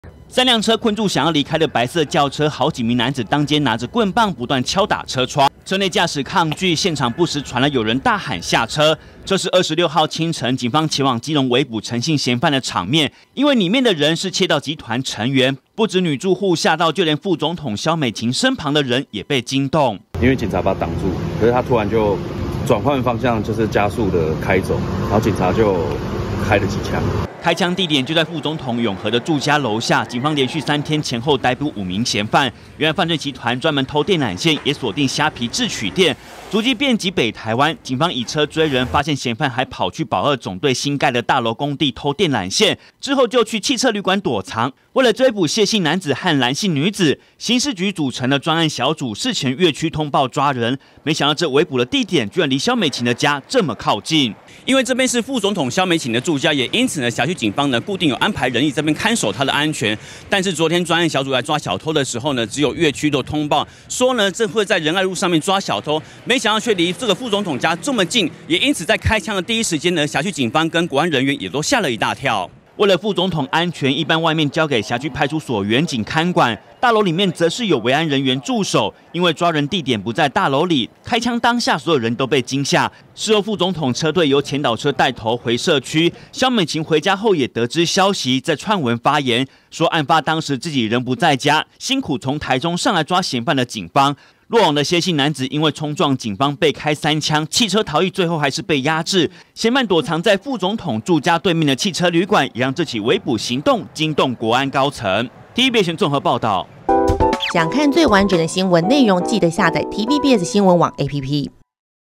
「仁愛路」抓賊 警圍捕開槍 驚動蕭美琴國安｜TVBS新聞
警方開槍圍捕竊盜集團，驚動副總統國安人員！其中謝姓男子和女同夥，得手贓物後，去永和的汽車旅館躲藏，專案小組以車追人，也事前通報轄區警方，果然，嫌犯開車衝撞，於是開槍追捕，當初，因為通報位置，只說「仁愛路」，沒想到頭尾總長一點二公里的路，卻正好在蕭美琴住家樓下。